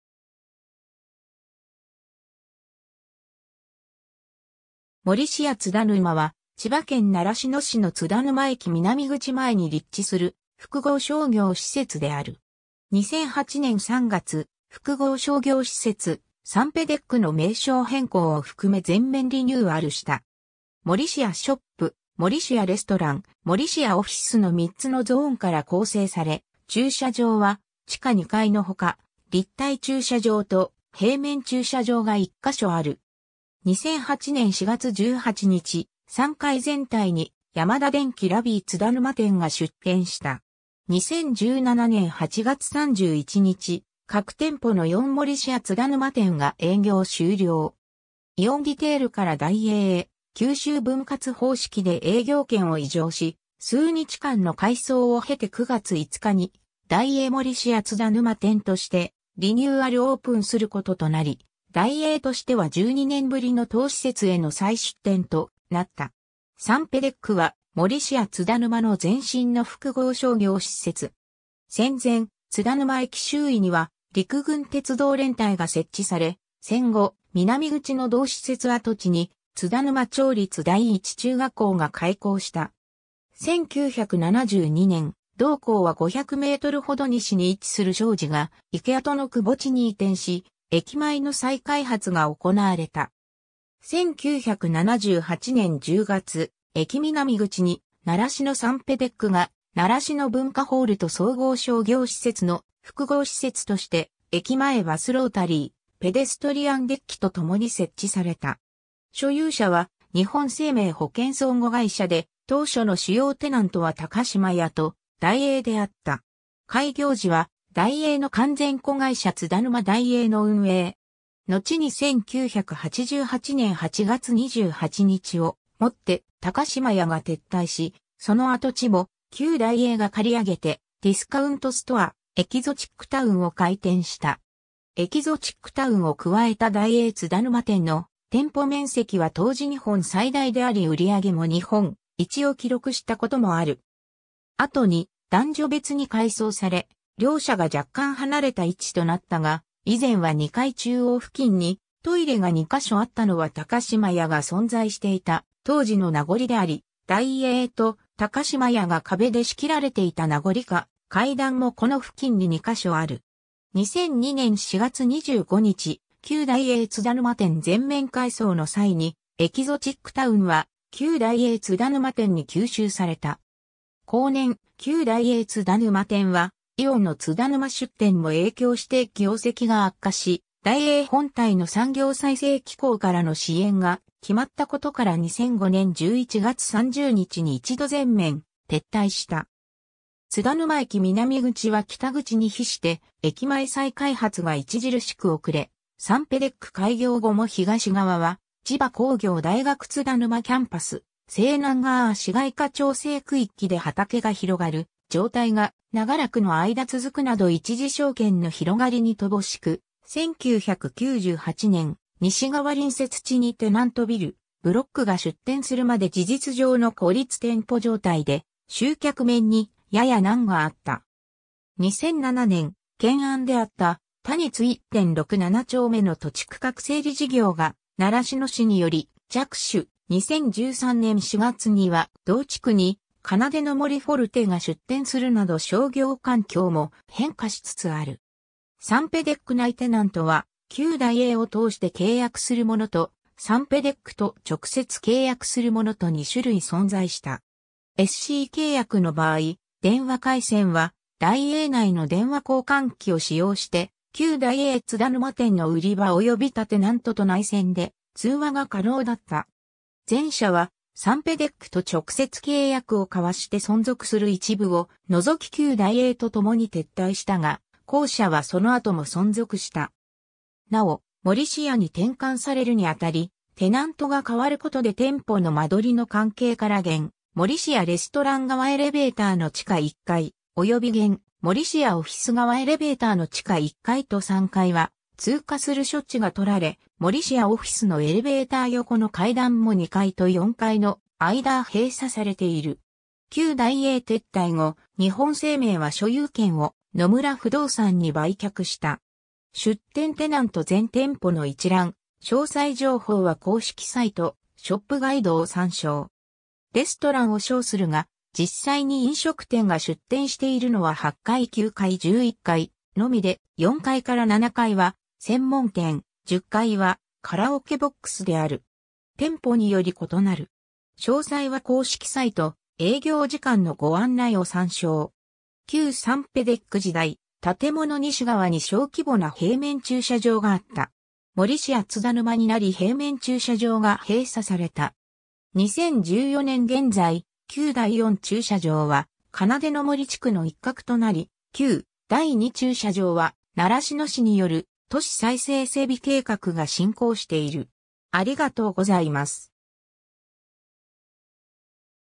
モリシア津田沼
モリシア津田沼（モリシアつだぬま、英称：Morisia Tsudanuma）は、千葉県習志野市の津田沼駅南口前に立地する複合商業施設である。
 2008年3月、複合商業施設「サンペデック」の名称変更を含め全面リニューアルした。
 Morisia Shop（商店街、B1階 - 3階）、Morisia Restaurant（レストラン街など、4階 - 11階）、Morisia Office（オフィス棟、4階 - 11階）の3つのゾーンから構成され、駐車場は地下2階のほか、立体駐車場（Morisia Shop2階に連絡通路）と平面駐車場（提携駐車場）が1箇所ある。
 2008年4月18日、3階全体にヤマダ電機 LABI津田沼店が出店した。
 2017年8月31日、核店舗のイオンモリシア津田沼店が営業を終了。
イオンリテールからダイエーへ吸収分割方式で営業権を移譲し、数日間の改装を経て9月5日にダイエーモリシア津田沼店としてリニューアルオープンすることとなり、ダイエーとしては12年ぶりの当施設への再出店となった。
 サンペデック（習志野サンペデック）は、モリシア津田沼の前身の複合商業施設。
 戦前、津田沼駅周囲には陸軍鉄道連隊が設置され、戦後、南口の同施設跡地に津田沼町立第一中学校（後の習志野市立第一中学校）が開校した。
1972年、同校は500mほど西に位置する庄司ヶ池跡のくぼ地に移転し、駅前の再開発が行われた。
 1978年10月、駅南口に「習志野サンペデック」が習志野文化ホールと総合商業施設の複合施設として、駅前バスロータリー、ペデストリアンデッキとともに設置された。
所有者は日本生命保険相互会社で、当初の主要テナントは髙島屋とダイエー（旧）であった。
開業時はダイエーの完全子会社津田沼ダイエーの運営。
のちに1988年8月28日をもって高島屋が撤退し、その跡地も旧ダイエーが借り上げてディスカウントストア「エキゾチックタウン」を開店した。
エキゾチックタウンを加えたダイエー津田沼店の店舗面積は当時日本最大であり売り上げも日本一を記録したこともある。
 後に男女別に改装され、両者が若干離れた位置となったが以前は2階中央付近にトイレが2か所あったのは高島屋が存在していた当時の名残でありダイエーと高島屋が壁で仕切られていた名残か階段もこの付近に2か所ある。
 2002年4月25日、旧ダイエー津田沼店全面改装の際にエキゾチックタウンは旧ダイエー津田沼店に吸収された。
後年、旧ダイエー津田沼店はイオンの津田沼出店も影響して業績が悪化しダイエー本体の産業再生機構からの支援が決まったことから2005年11月30日に一度全面撤退した（ダイエーはのちにイオングループ傘下となる）。
 津田沼駅南口は北口に比して駅前再開発が著しく遅れ、サンペデック開業後も東側は千葉工業大学津田沼キャンパス、西南側は市街化調整区域で畑が広がる状態が長らくの間続くなど一次商圏の広がりに乏しく、1998年、西側隣接地にテナントビル「ブロック」（現・Loharu津田沼）が出店するまで事実上の孤立店舗状態で集客面にやや難があった。
2007年、懸案であった谷津1・6・7丁目の土地区画整理事業（「奏の杜プロジェクト」。前出の、かつて畑が広がっていた西南側）が習志野市により着手、2013年4月には同地区に「奏の杜forte（フォルテ）」（キーテナントはベルク）が出店するなど商業環境も変化しつつある。
 サンペデック内テナントは旧ダイエーを通して契約するもの（SC契約）と、サンペデックと直接契約するものと2種類存在した。
SC契約の場合、電話回線はダイエー内の電話交換機を使用して旧ダイエー津田沼店の売り場および他テナントと内線で通話が可能だった。
前者はサンペデックと直接契約を交わして存続する一部（アニメイト津田沼店など）を除き旧ダイエーとともに撤退したが、後者はその後も存続した。
なお、モリシアに転換されるにあたりテナントが変わることで店舗の間取りの関係から現 Morisia Restaurant 側エレベーターの地下1階、および現 Morisia Office 側エレベーターの地下1階と3階は通過する処置がとられ、Morisia Office のエレベーター横の階段も2階と4階の間は閉鎖されている（正確には地下2階から4階の部分まで階段自体は続いてはいるものの3階は売り場...